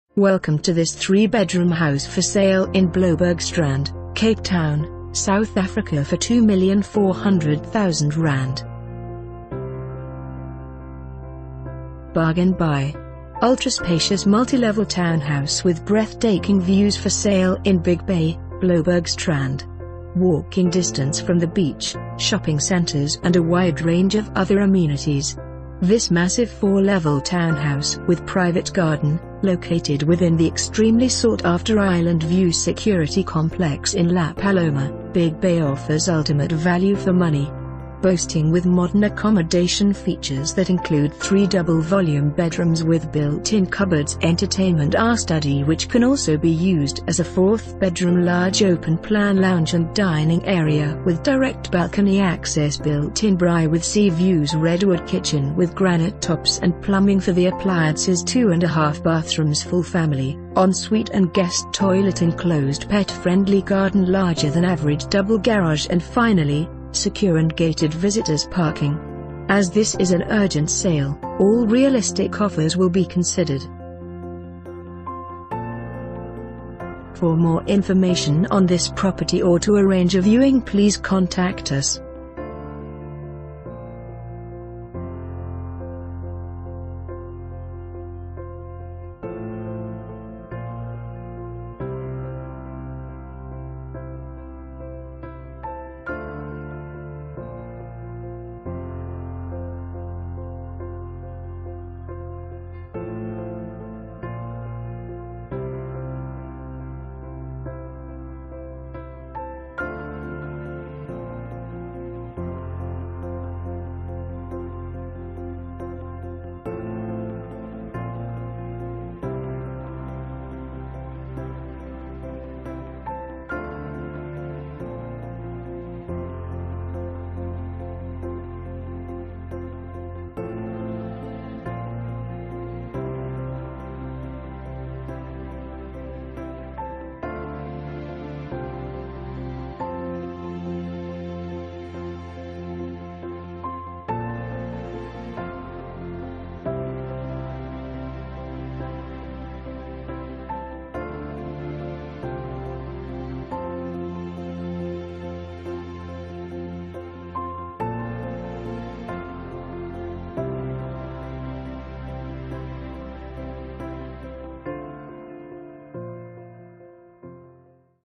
3 Bedroom House For Sale in Bloubergstrand, Cape Town, South Africa for ZAR 2,400,000...
Bargain Buy! Ultra-spacious “multi-level” townhouse with breath taking views for sale in Big Bay - Bloubergstrand!
Walking distance from the beach, shopping centres and a wide range of other amenities! This massive 4 level townhouse with private garden, located within the extremely sought after Island View security complex ...